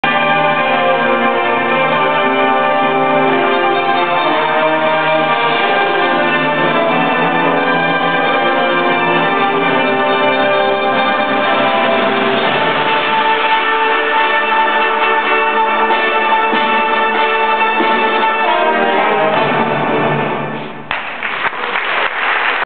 Splendour of Brass
Splendour of Brass, brought to Perth by Western Australia Symphony Orchestra. Featuring James Ledger and Sachio Fujioka.